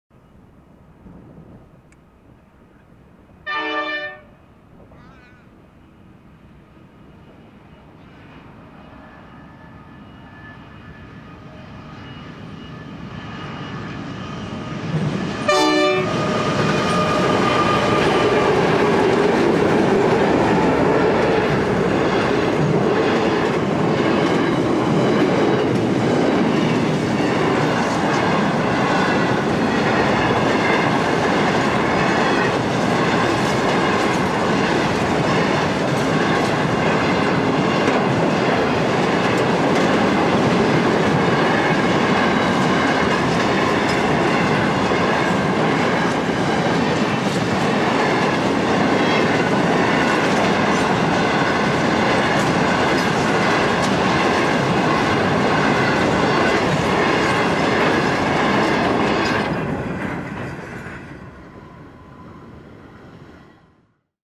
XM4 Pacific National Steel Train - PoathTV Australian Railways
Pacific National steel service XM4 (9822) operating between Port August (SA) and Melbourne (Vic).

Locomotives: NR80 + X50 + 8124 + BL33